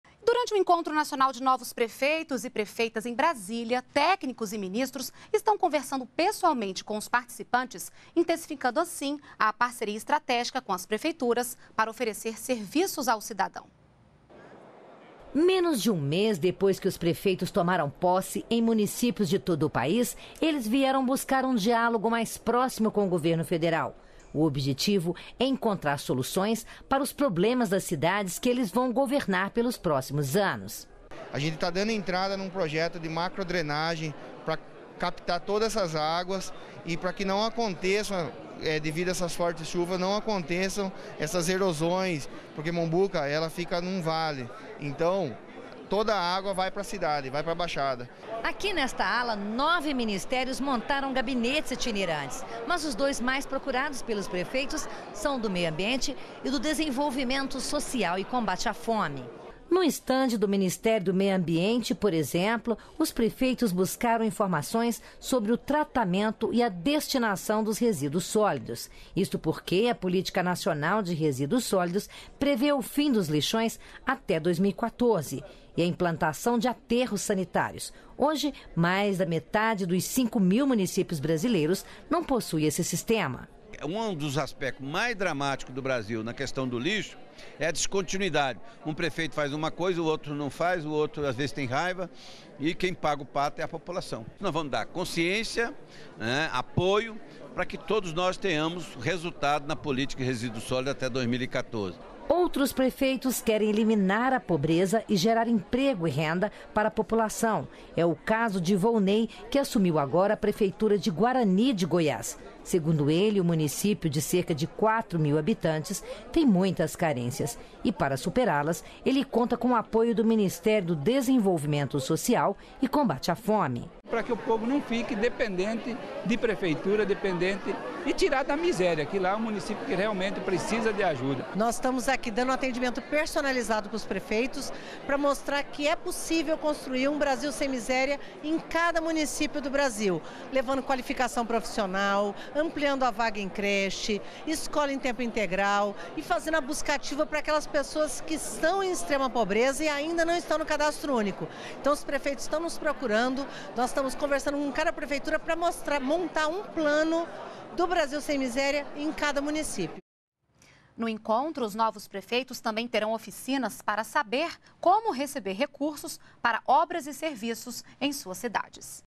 Prefeitos recebem orientação de técnicos e ministros em Brasília
NBR NOTÍCIAS - 29.01.13: Os participantes do Encontro Nacional de Novos Prefeitos e Prefeitas conversam com ministros e técnicos de diversas áreas para buscar informações e soluções para cada município. A assistência tem o objetivo de intensificar a parceria estratégica com as prefeituras e oferecer melhores serviços a população.